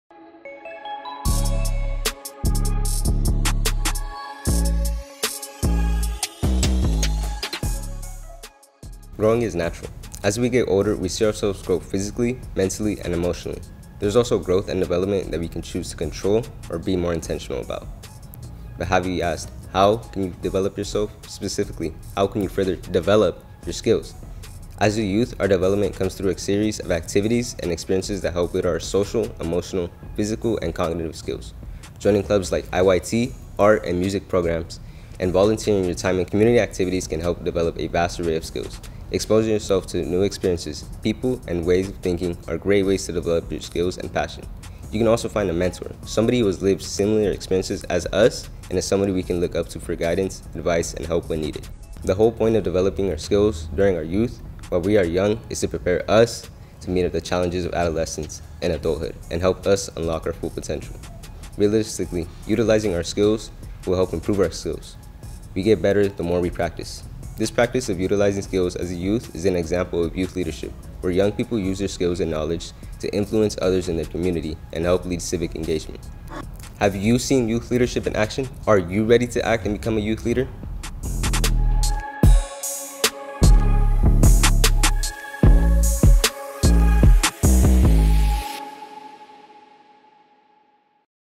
A Brothers Guide to Organizing, Chapter 6: Youth Development and Leadership with Juan Pablo Reyes
In this preface, IYT Brother Juan Pablo Reyes shares how youth can develop their skills to help reach their personal goals and prepare for the challenges that come with adulthood.